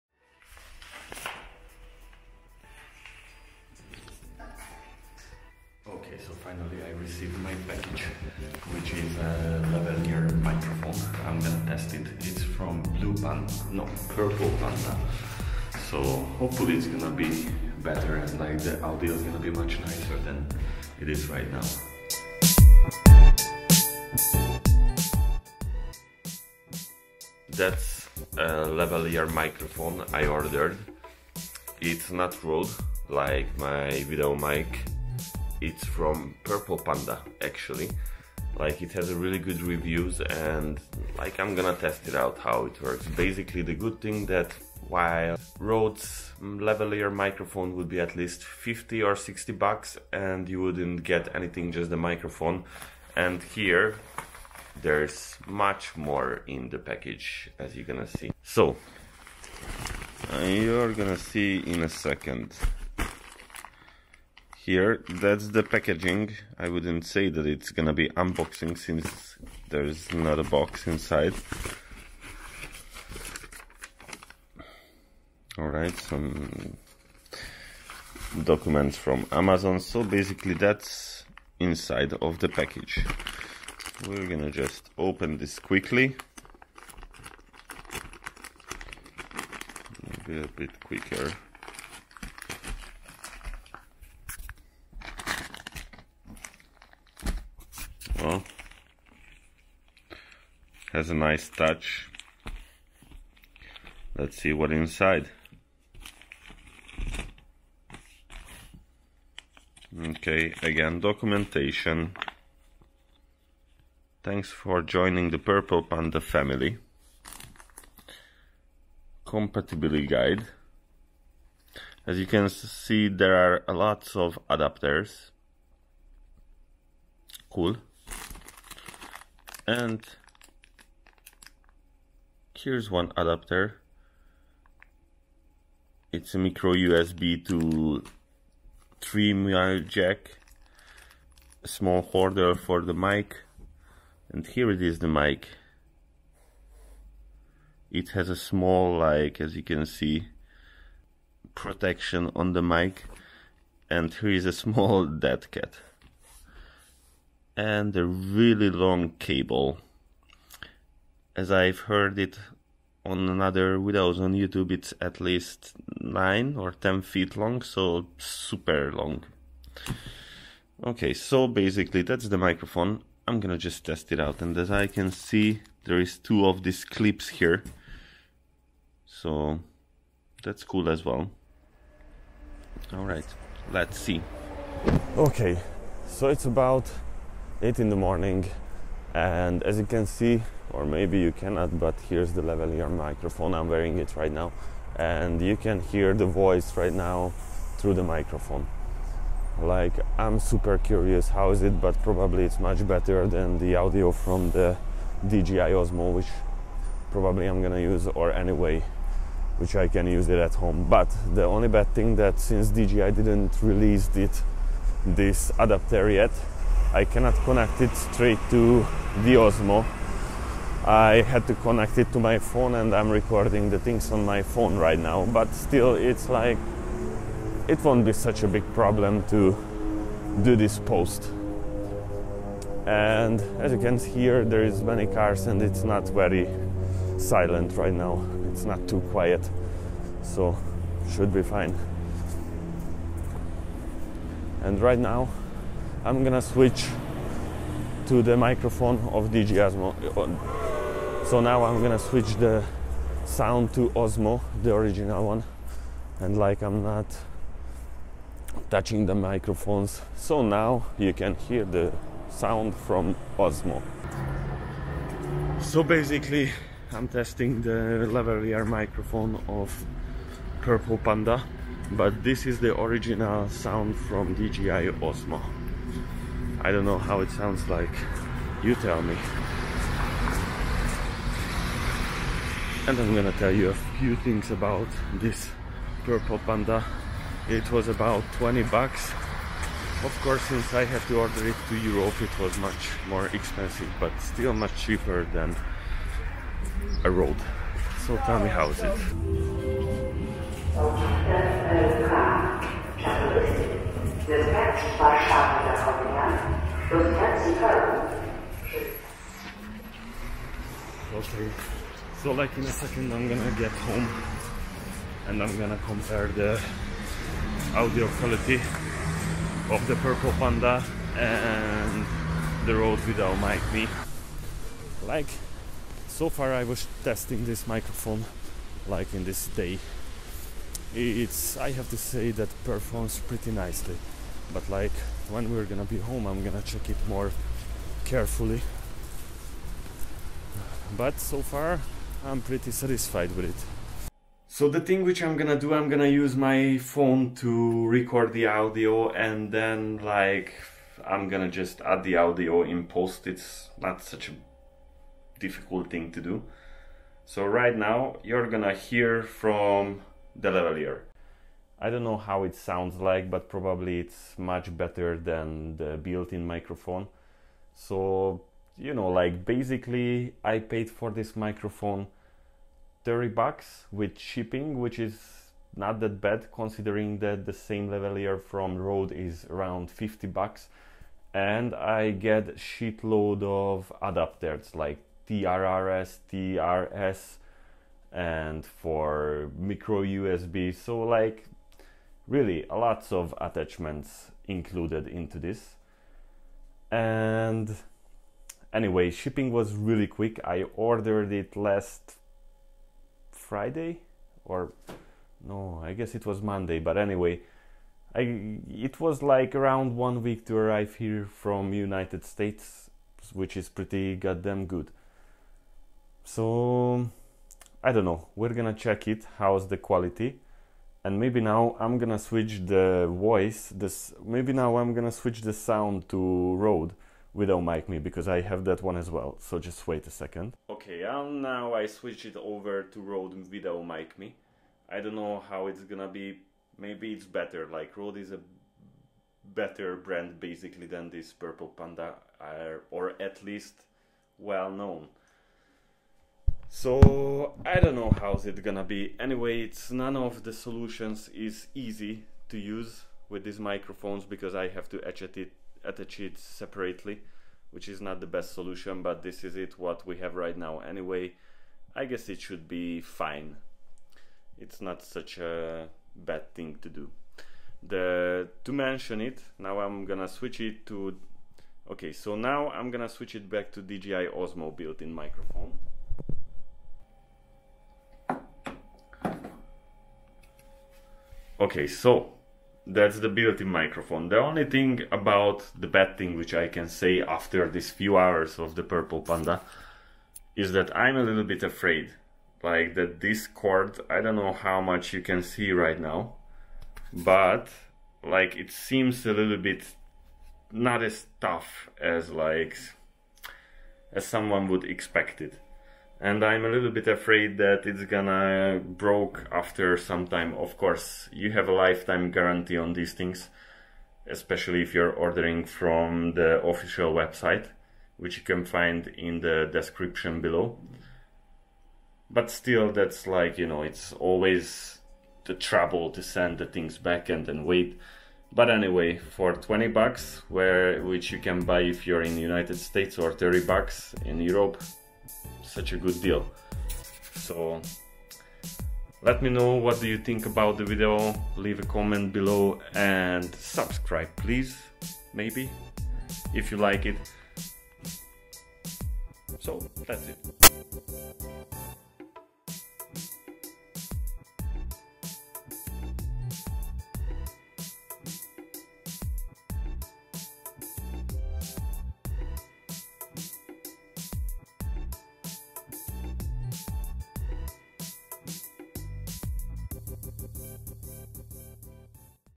BEST CHEAP LAVALIER MIC?! Purple Panda Review
I've tested out Purple Panda's lavalier microphone. For it's price it is a great deal. I'm very satisfied with the product!

Purple Panda

Rode Video Mic ME

Dji Osmo Pocket